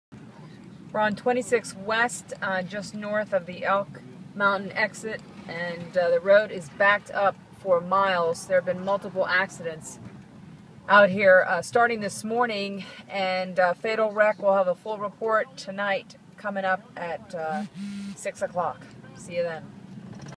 Fatal wreck WLOS I-26
Just n of woodfin tune in at 6pm full details woman in her mid twenties has died